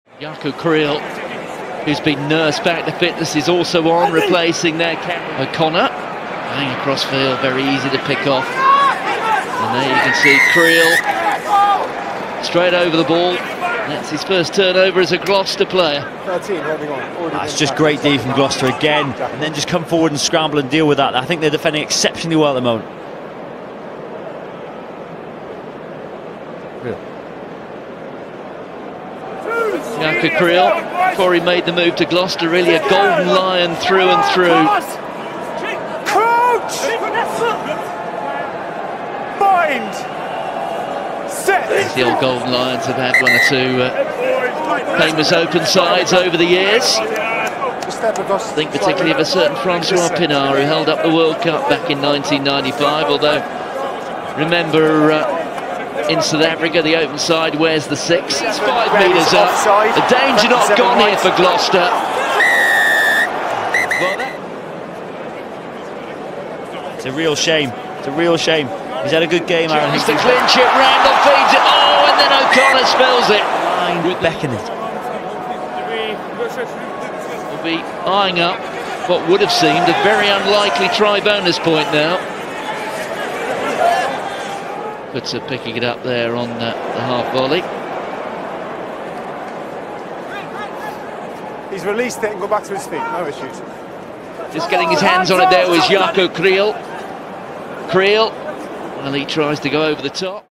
Jaco Kriel Debut vs Bristol Bears (02/11/2018) HD By CROSE
Good Turnovers & Carry! Excited to see him make his full Prem debut soon!
Credit to RugbyPass for the footage!